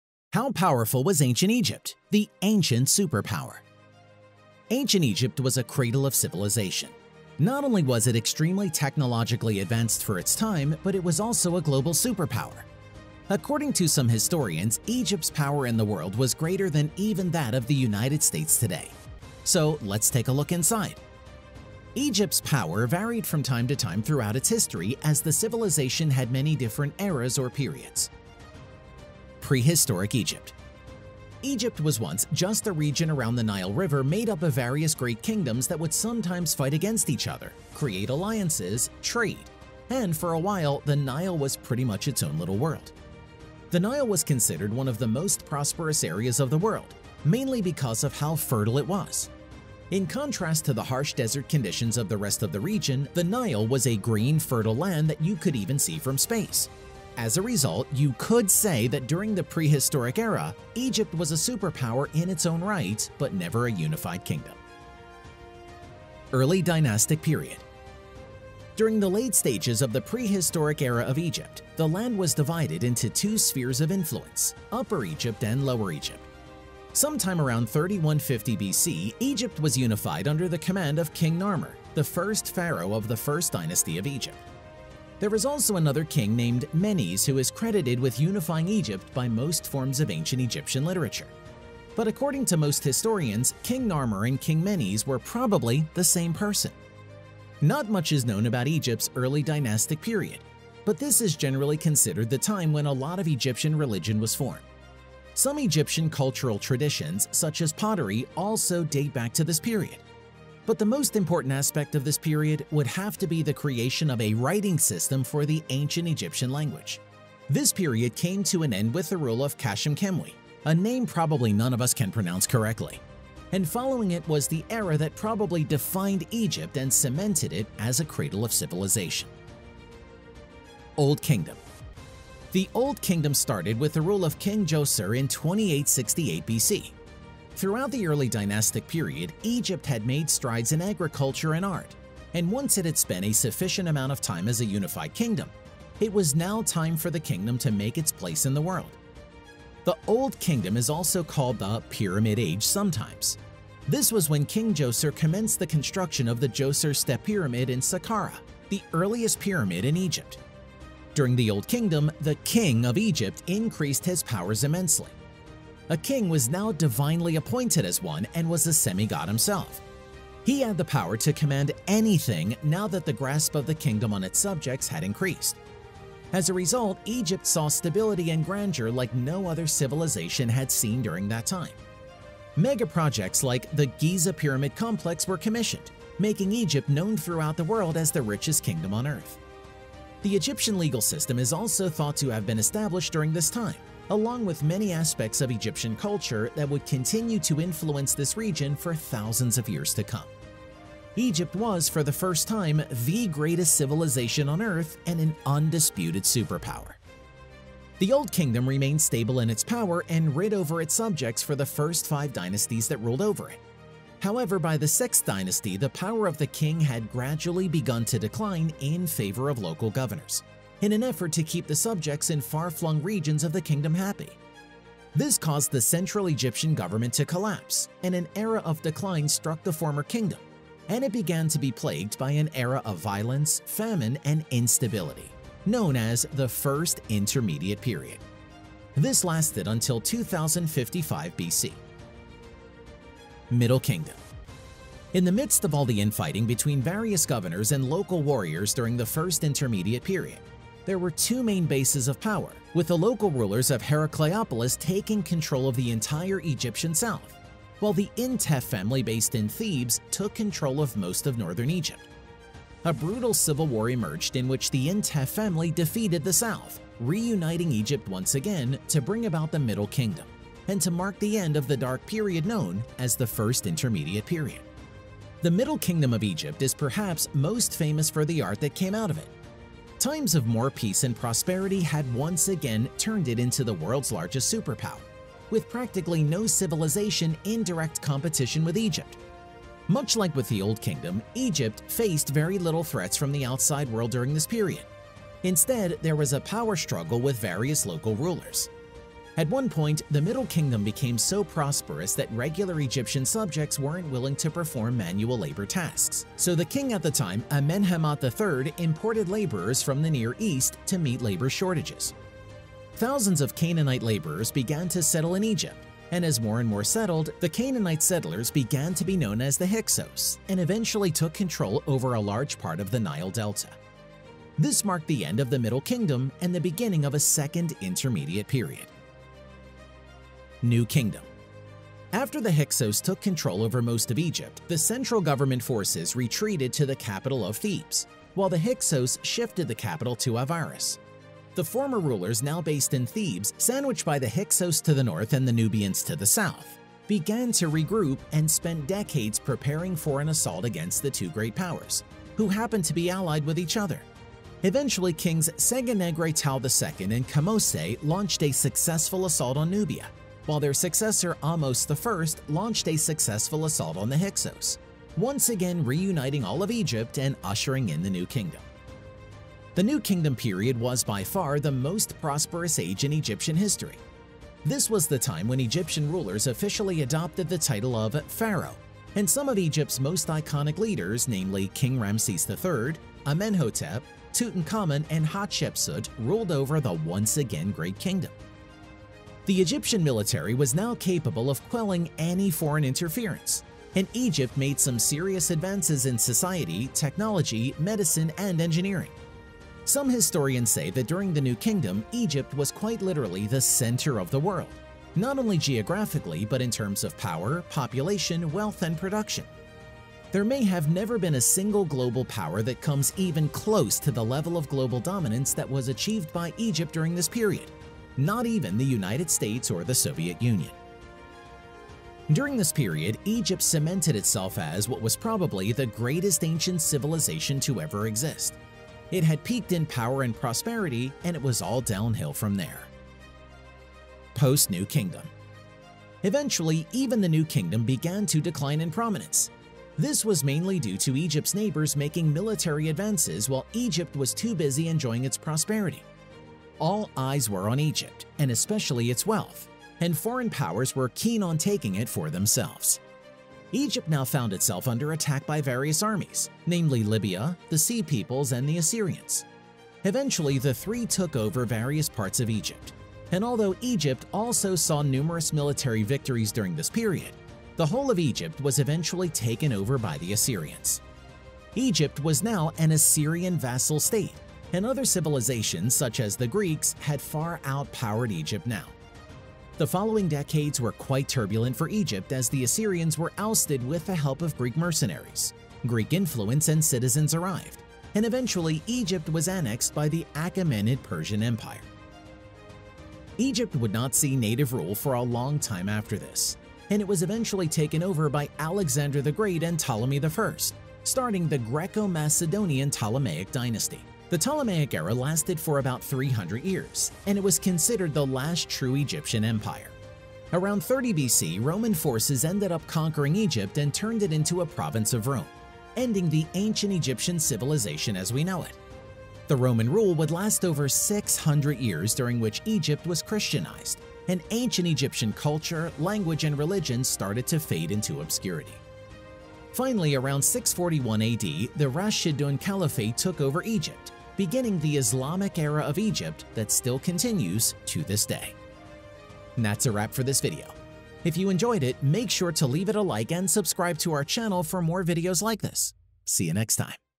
How POWERFUL Was Ancient Egypt? The Ancient Superpower
Out of all the ancient superpowers in history, ancient Egypt seemed to be the one that dominated everything due to their incredible advancements in all aspects of society.